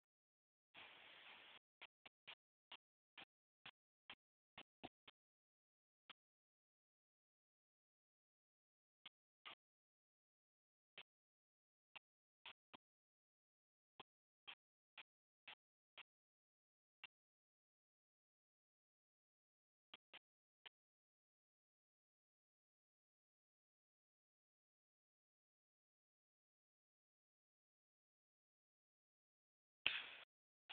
My Last Hour with Duke, our poodle.
Our dog duke passed away due to an auto immune disease and eventually developing pnuemonia.